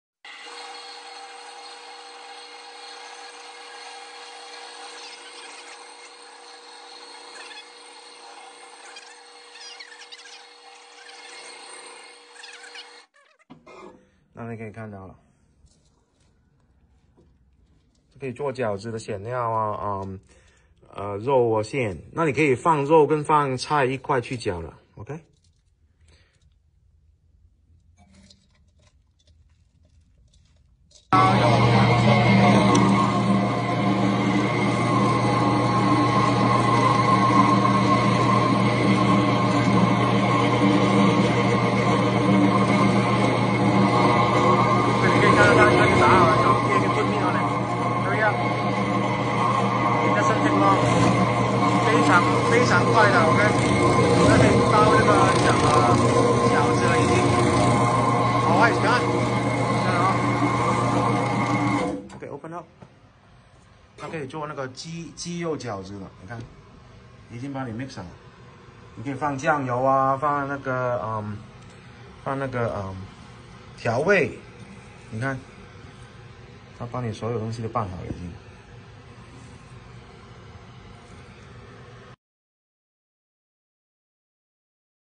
Buffalo Chopper Food Processor by CCONLY RED (Chinese language)
This video is demonstration of our Buffalo Chopper Food Processor chopping chicken and vegetables fillings for dumplings.